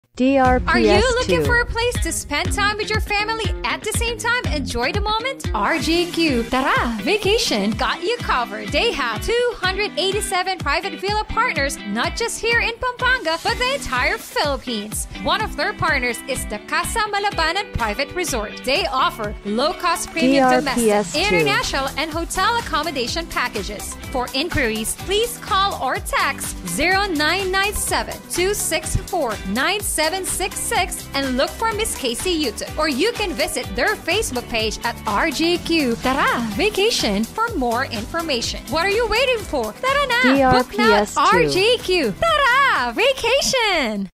RJQ Travel Services Radio Ad 2023 44s (Angeles, Pampanga)
Ito po ang Q4 2023 Provincial Radio Ad ng RJQ Travel Services sa Angeles, Pampanga.

DRPS Two 5732: RJQ Travel Services Radio Ad 2023 44s (Angeles, Pampanga)